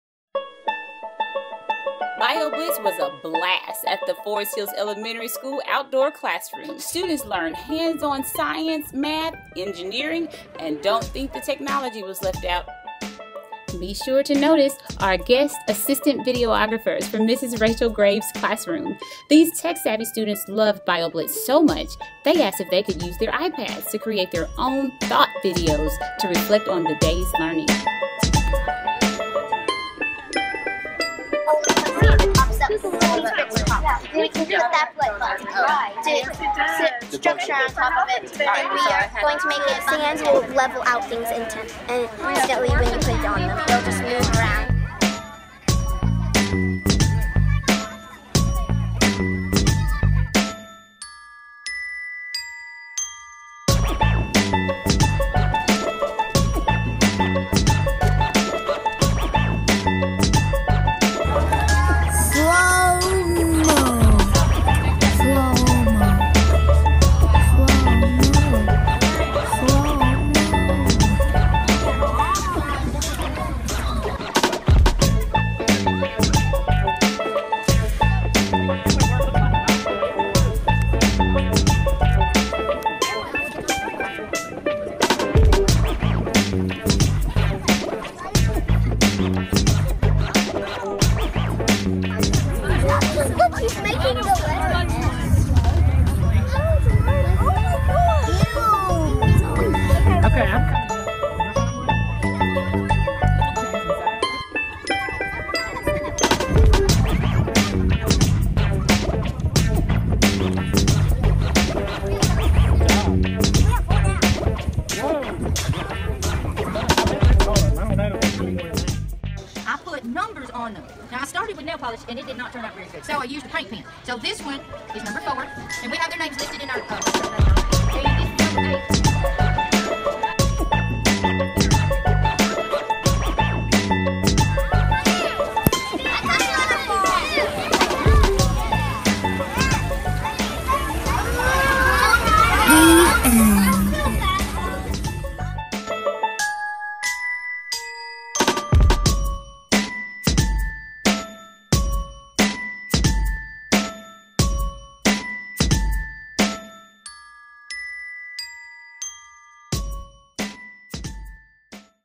"BIO BLITZ!": See the Outdoor Classroom @Forest Hills Elementary
See Forest Hills Elementary students, teachers & parents, alongside community supporters, work and learn together as they help their "Outdoor Classroom" come to life!